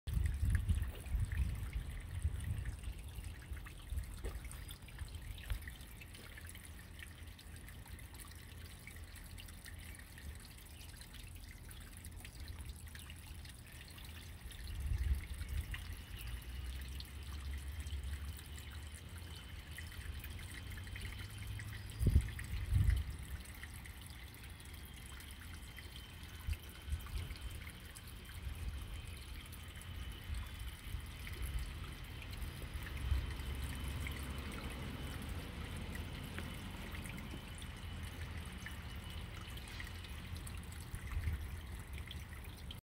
pond 2021 have flower blooms!
first time I have seen the water iris bloom and water hyacinths are blooming!